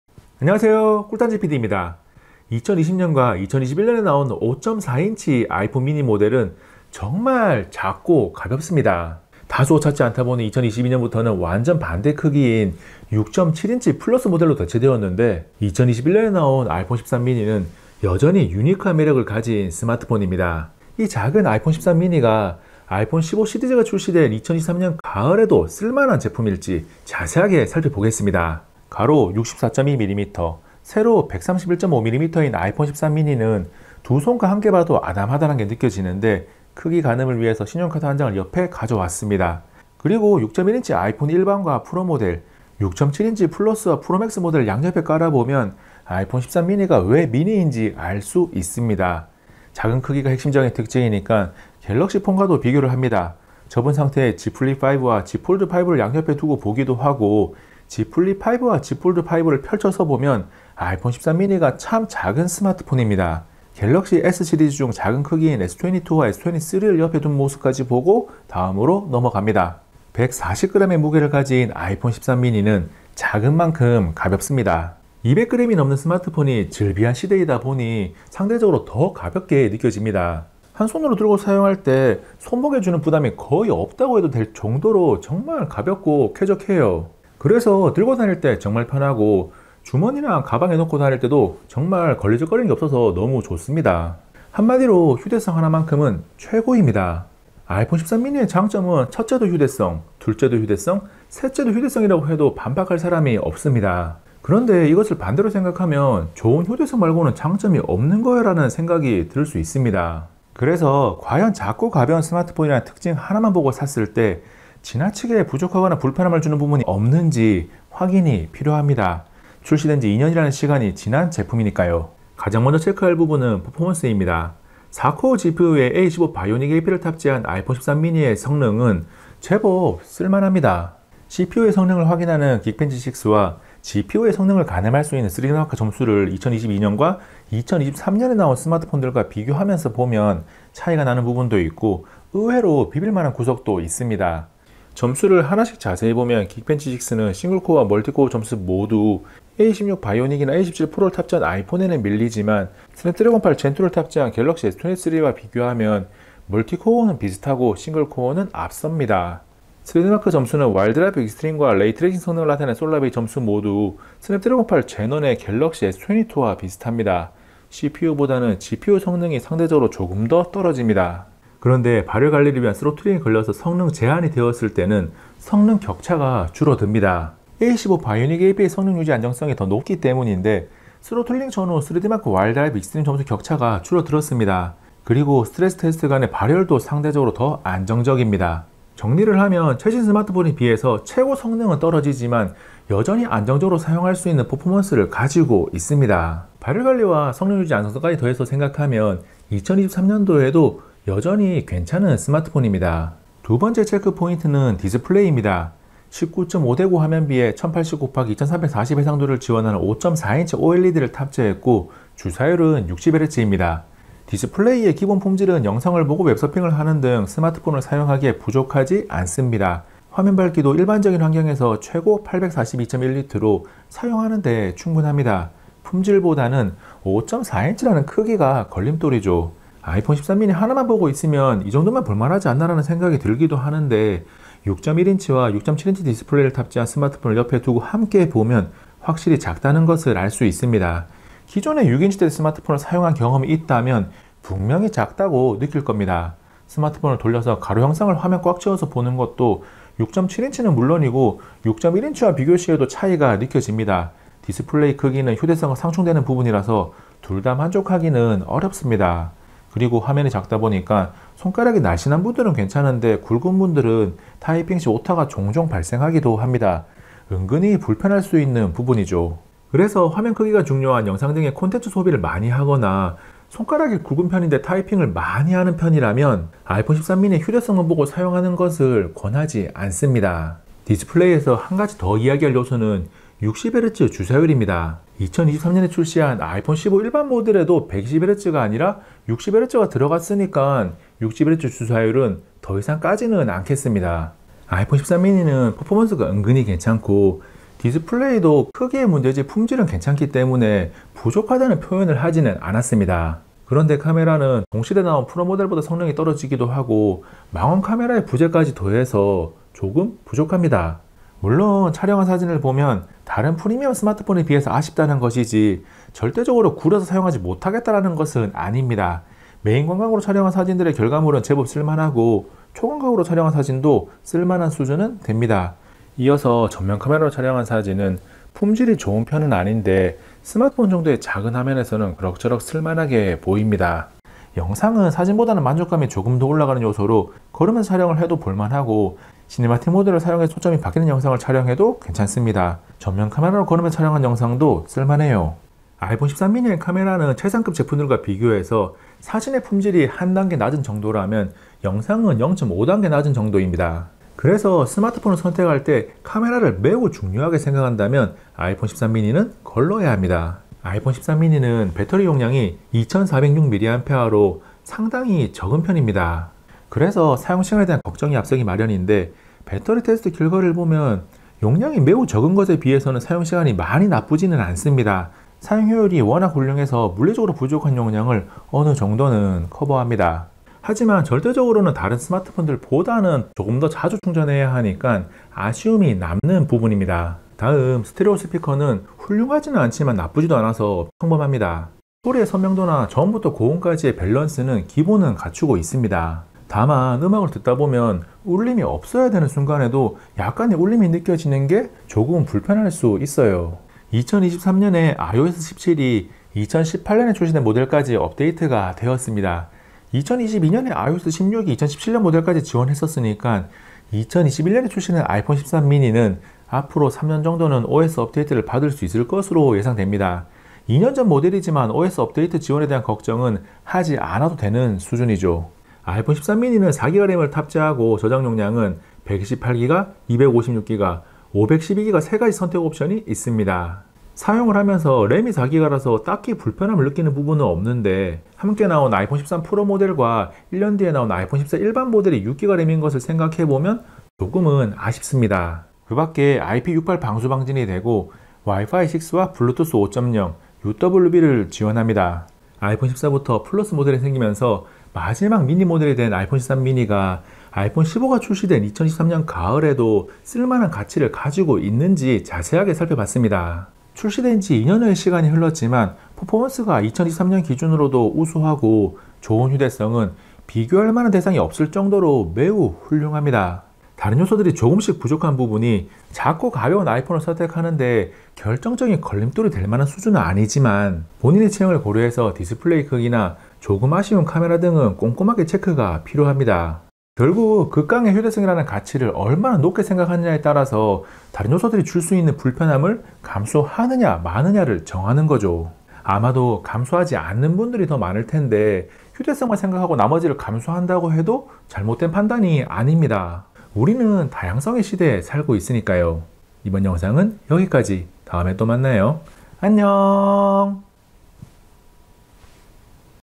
작고 가벼운 아이폰 13 미니, 아이폰 15 나온 지금도 쓸만할까? (2023년 가을)
아이폰 15가 출시된 2023년 가을에 2021년에 출시된 5.4인치 스마트폰 아이폰 13 미니가 쓸만한 가치가 있는지 자세하게 살펴봤습니다. 아이폰 13 미니는 작고 가벼운 마지막 미니 모델 입니다.

📌 타임 스탬프
0:00 아이폰 13 미니
0:28 크기
1:09 무게
2:02 퍼포먼스
2:51 쓰로틀링
3:23 디스플레이
5:03 카메라
6:00 배터리
6:27 스피커
6:43 OS 업데이트
7:32 꿀PD 정리

구독을 누르면 꿀단지PD 영상을 빠르게 만나볼 수 있어요!